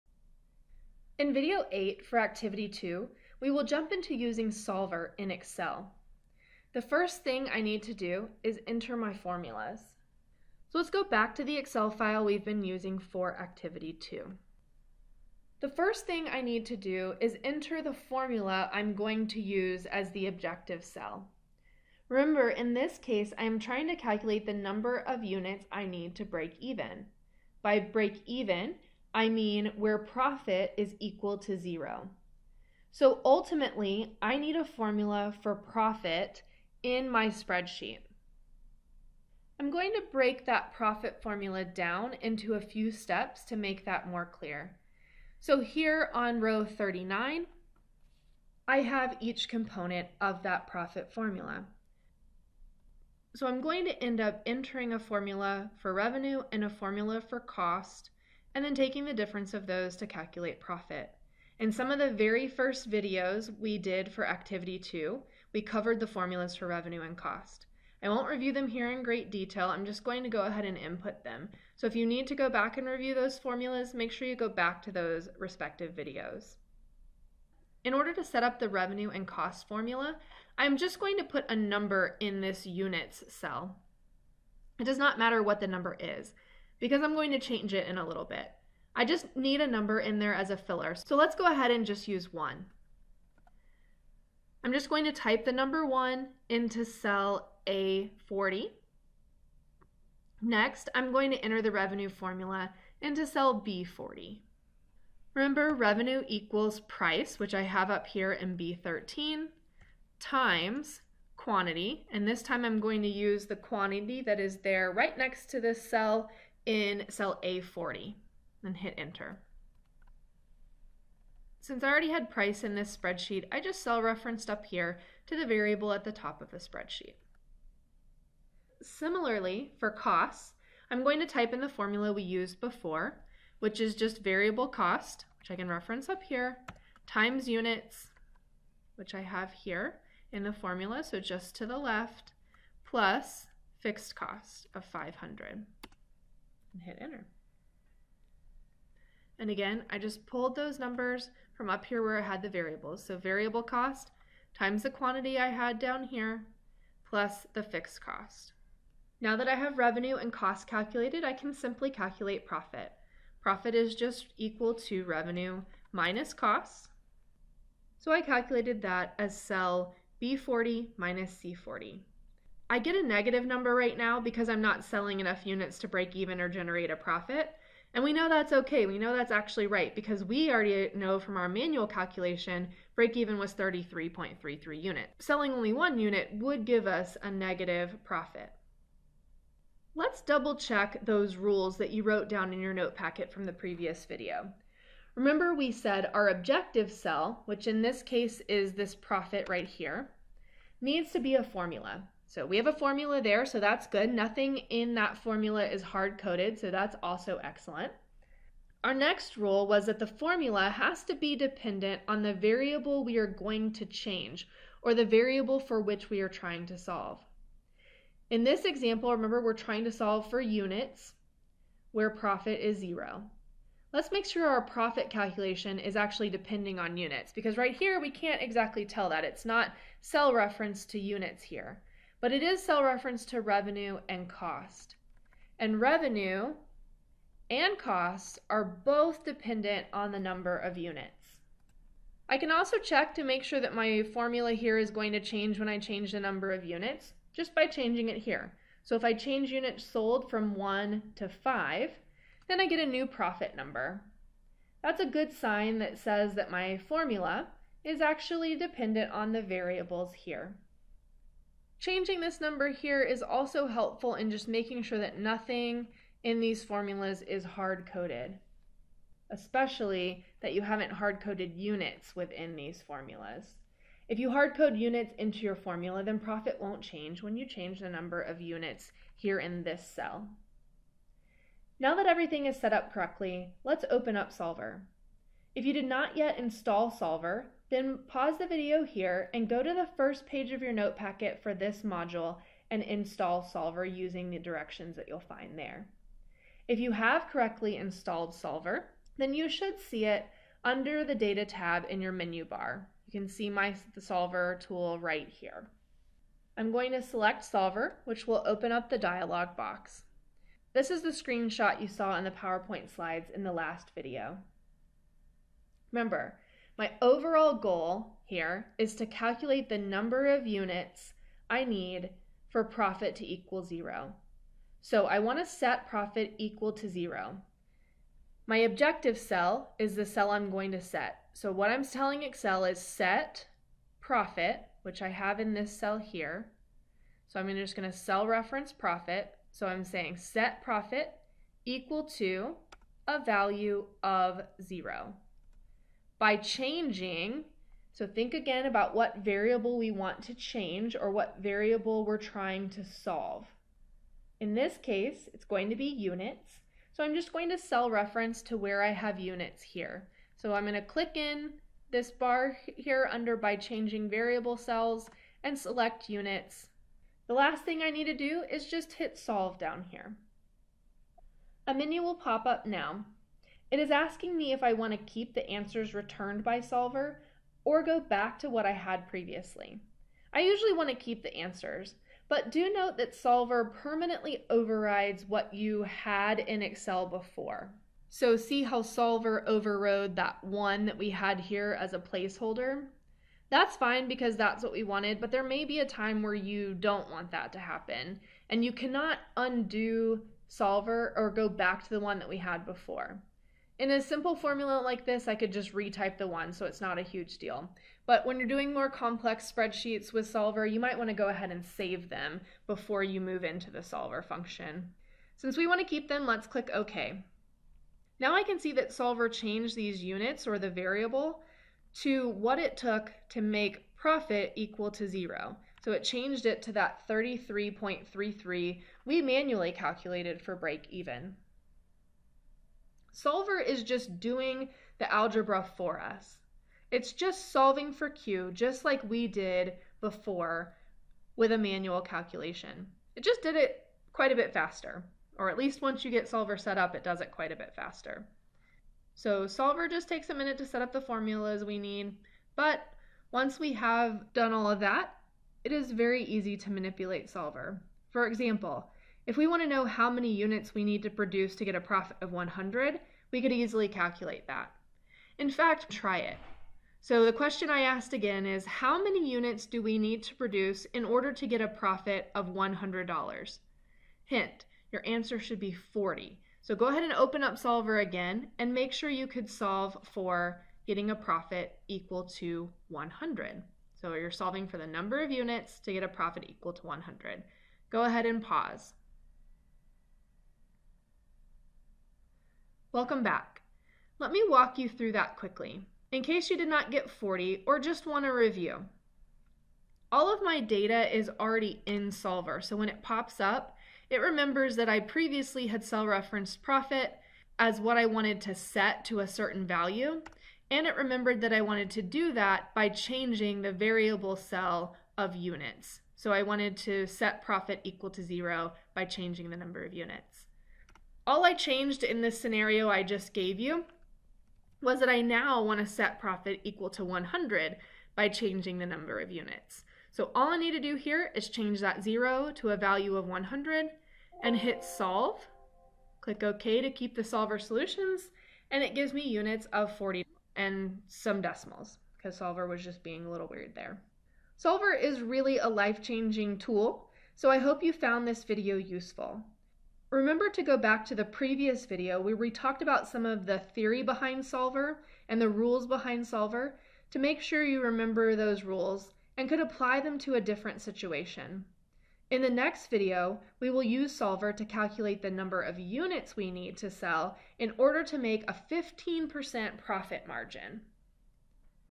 Mays MBA Prep Business Math and Excel Activity 2 Video 8
Calculating breakeven using Excel Solver